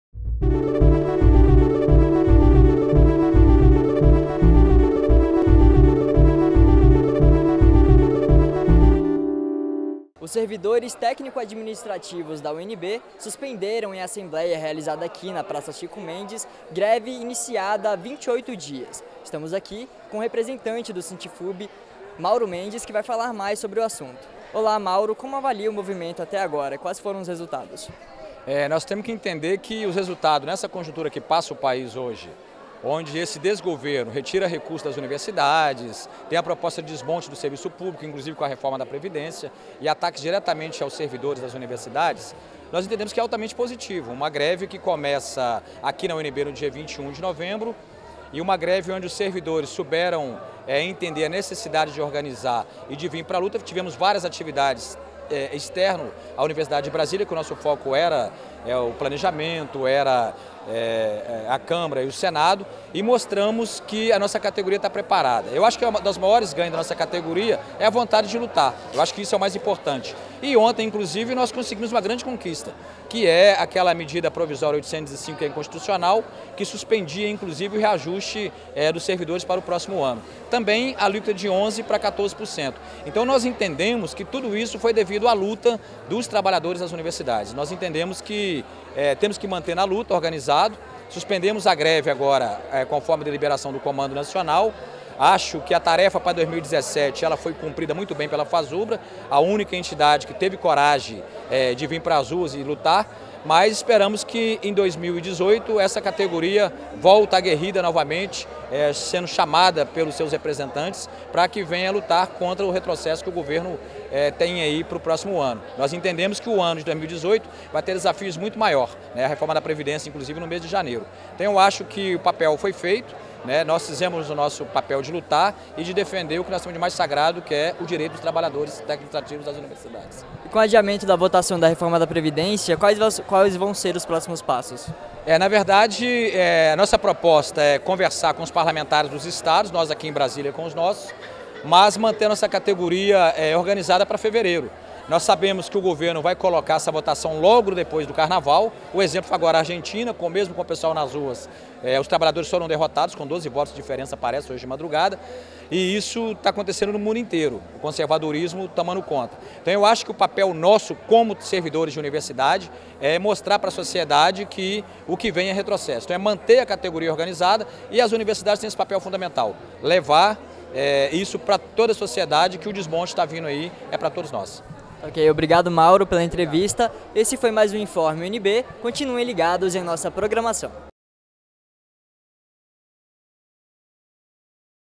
Informe: Servidores da UnB suspendem greve
A categoria estava paralisada há 28 dias. A suspensão se deve ao adiamento da votação da reforma da previdência para fevereiro. Confira!

UnBTV - Dezembro/2017.